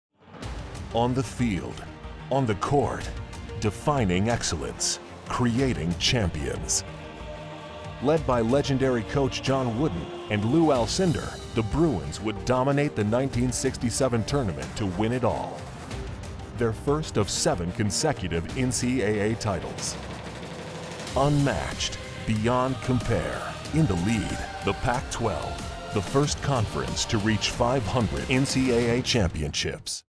First to 500: 1967 UCLA men's basketball
John Wooden led UCLA men's basketball to the first of seven consecutive NCAA titles in 1967. It was the 78th title for the Pac-12.